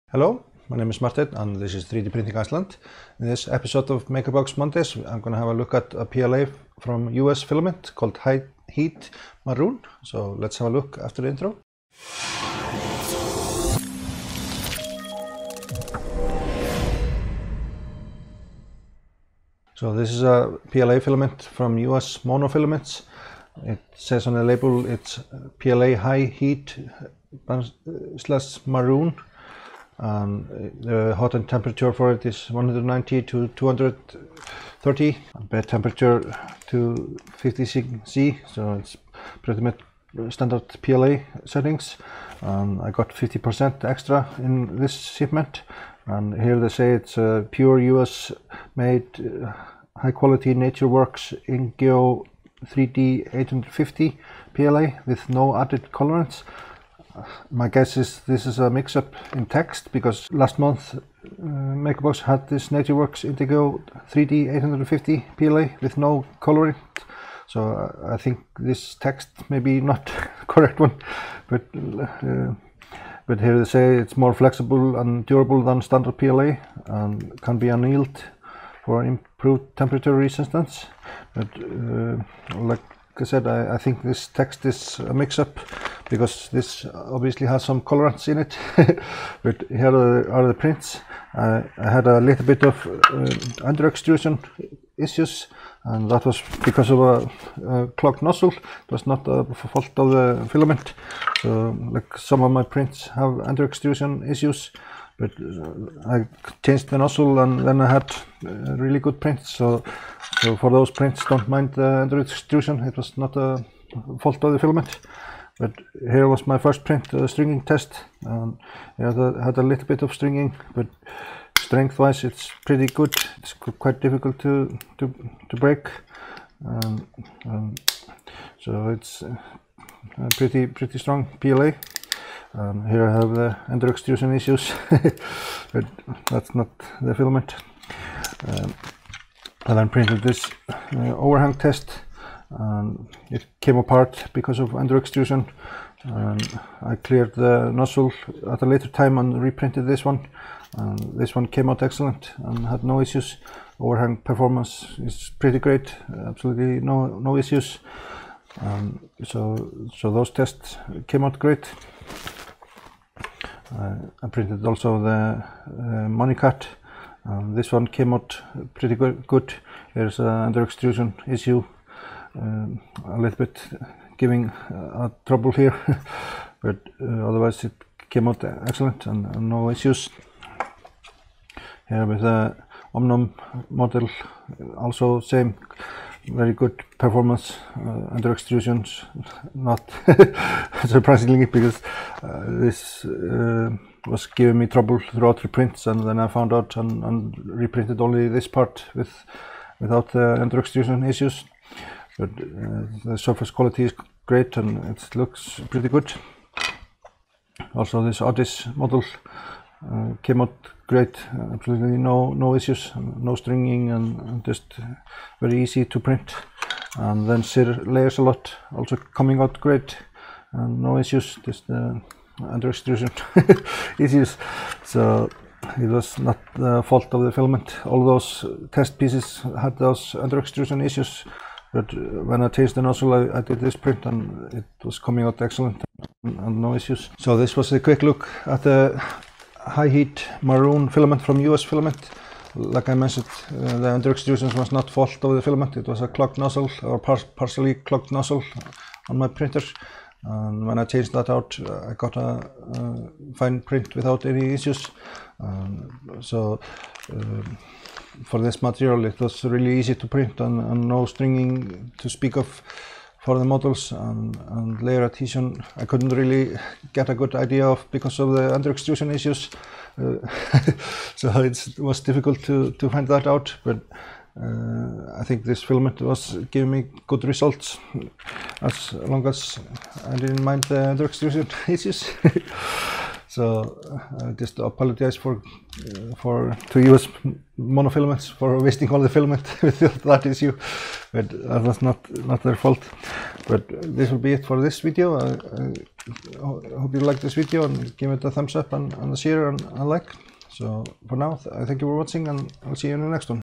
Makerbox Monday - US Monofilaments PLA High Heat
In this video I test out a makerbox sample of US Monofilaments PLA High Heat